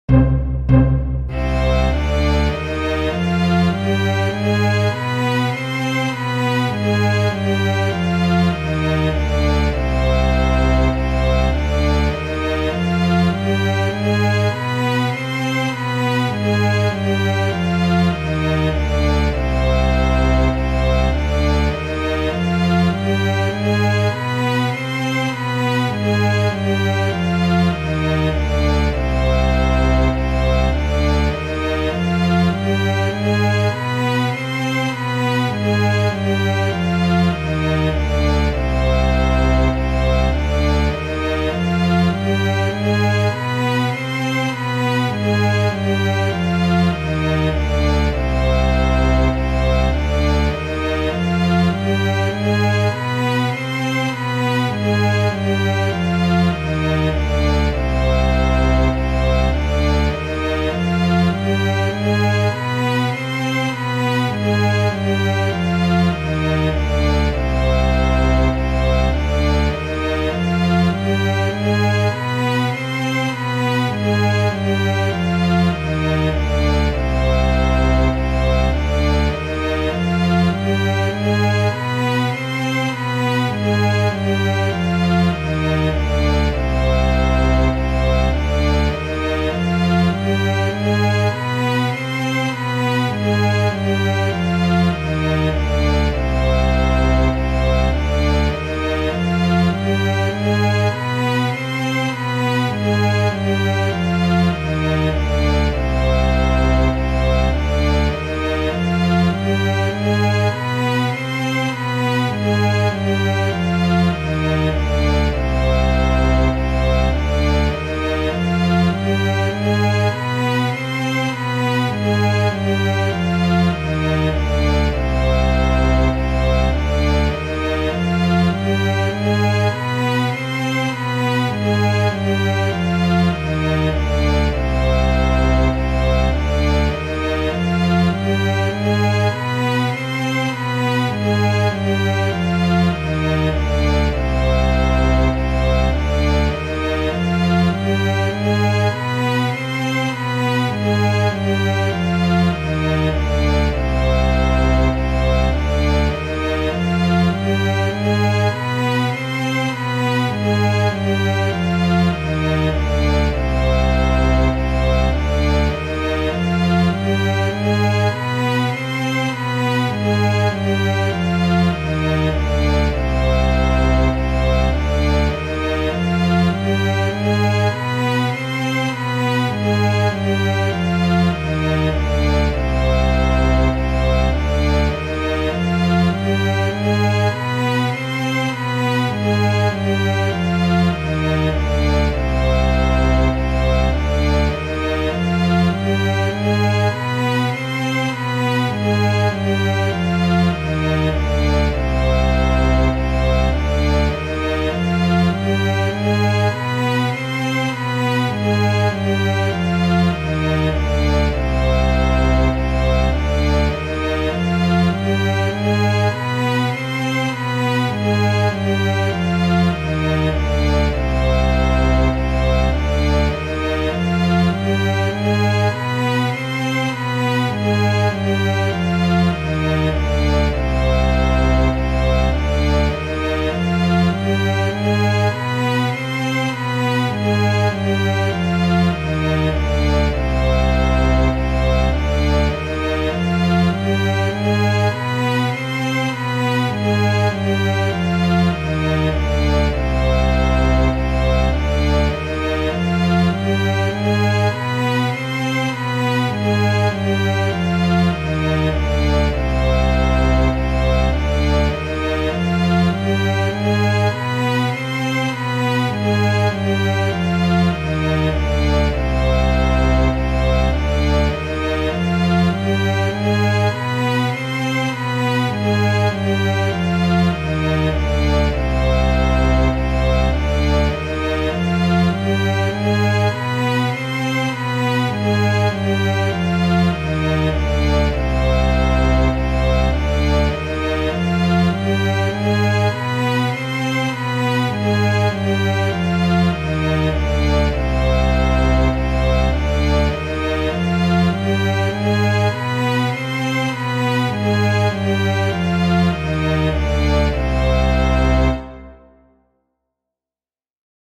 C Harmonic Minor Scale One Octave at 100bpm Backing Track Color
Play or sing along. This track features double bass, cello, viola and violin playing C Harmonic Minor Scale One Octave at 100bpm. Sheet music is provided on G treble clef, C alto clef and F bass clef.